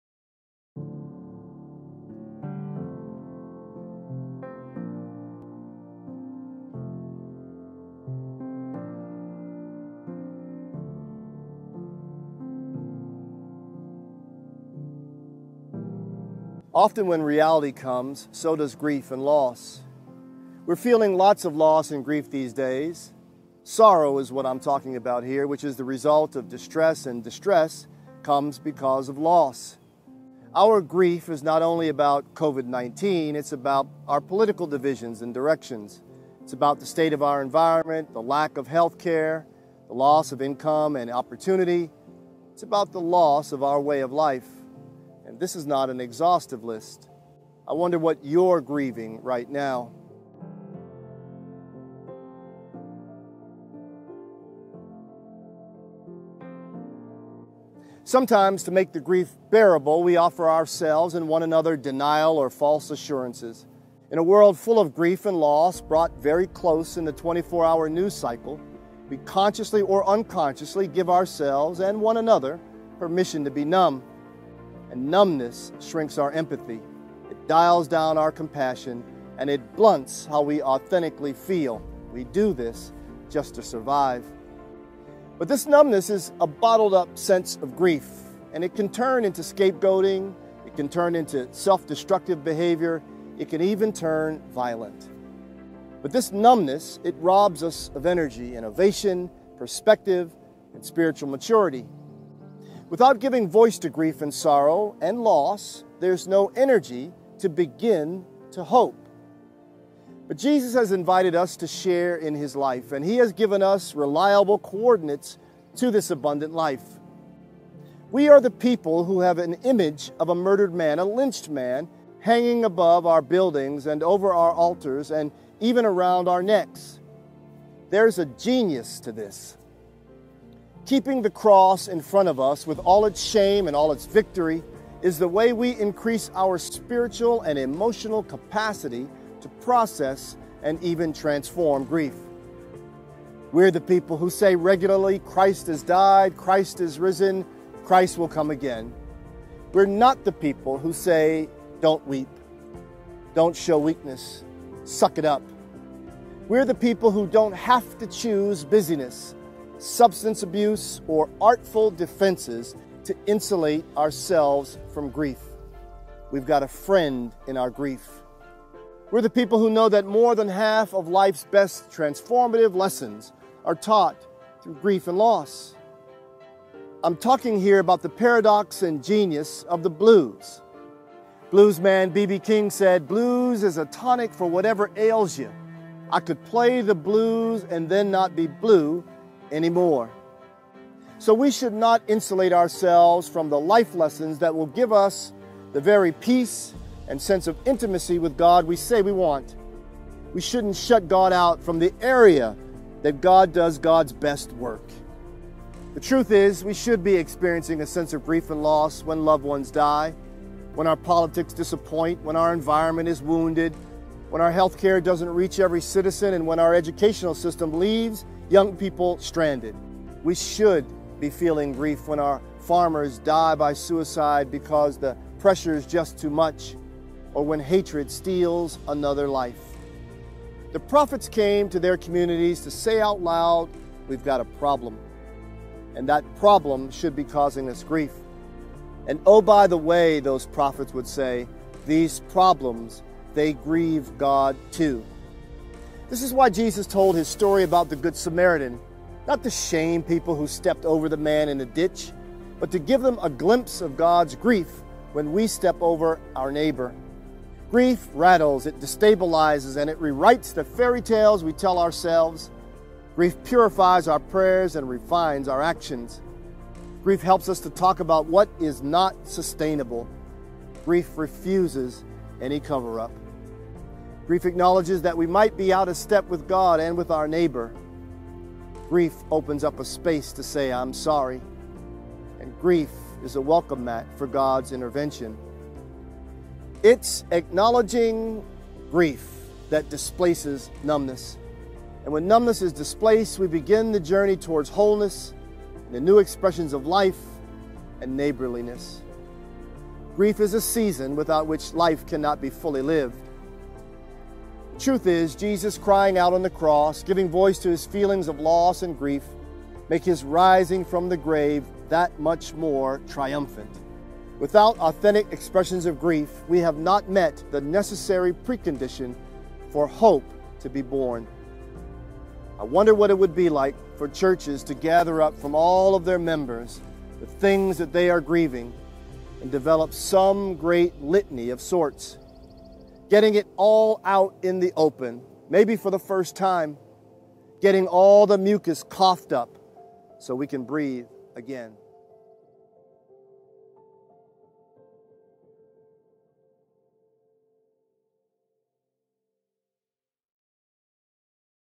Movements of Faith | Part 2 of 5: Grief
In life, we go through movements. These movements change in each season, and during this season these movements within ourselves may feel impossible to overcome. Join Bishop Wright as he takes a deeper dive into what these movements look and feel like as a people of faith.